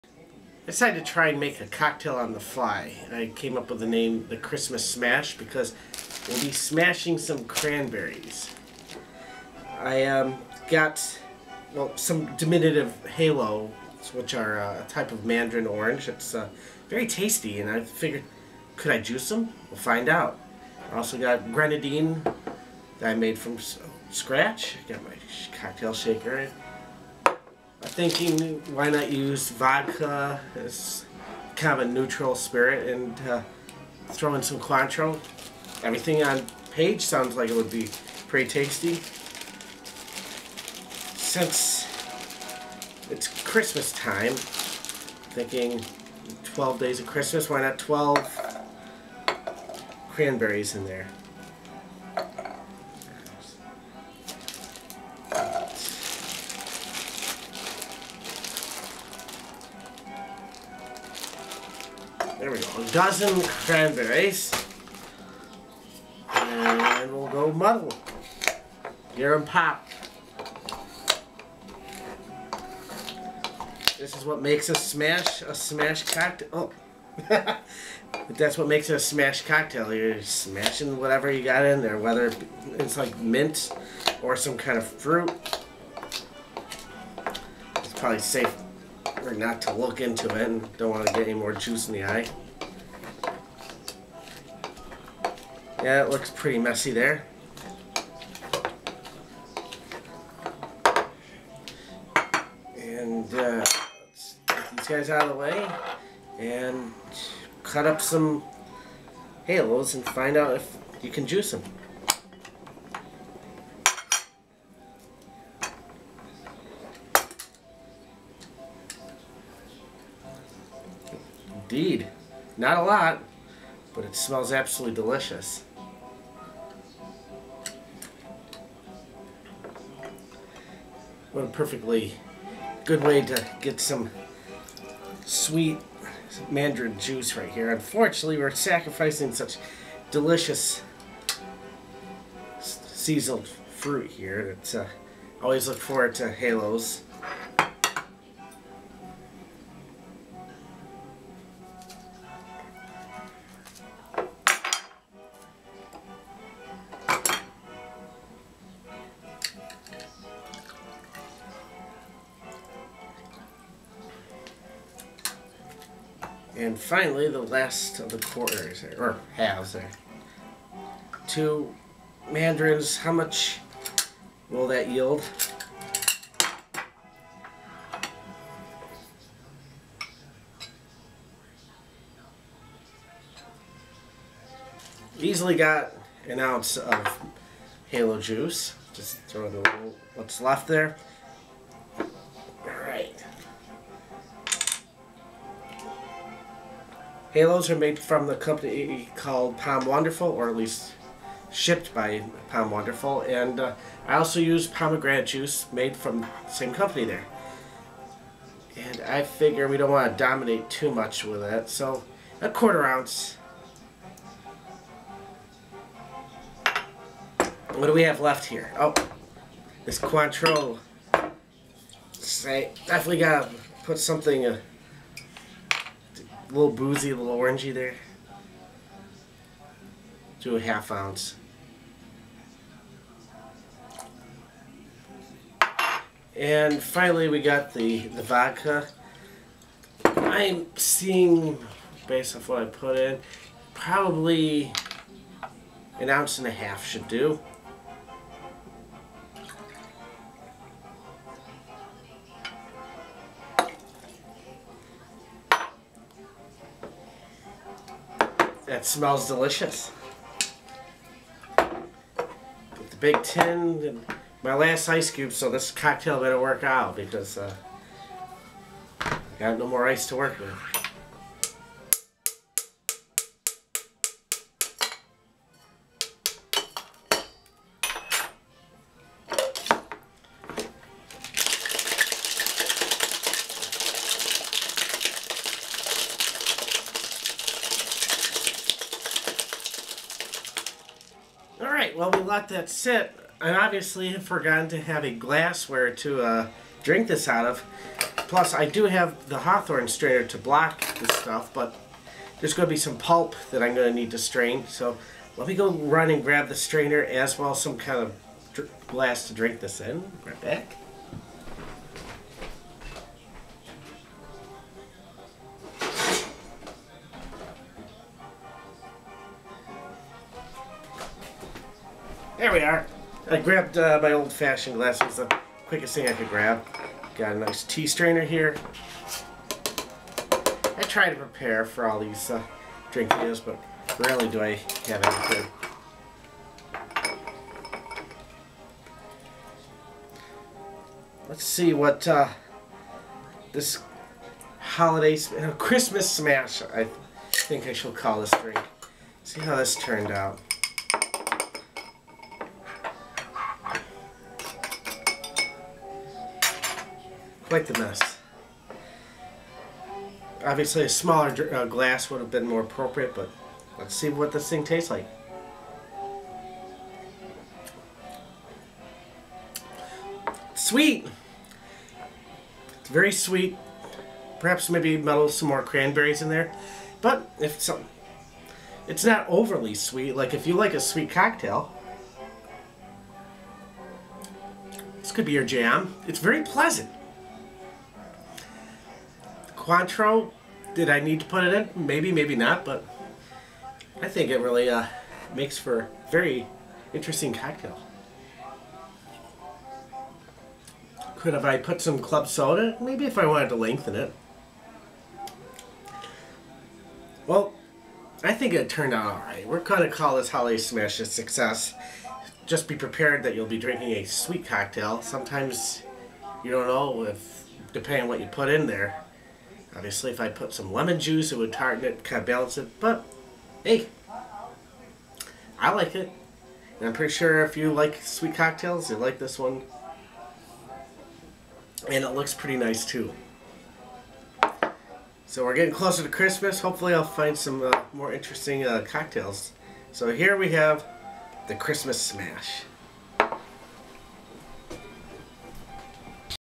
Christmas Smash Cocktail
Making a sweet drink with seasonal flavors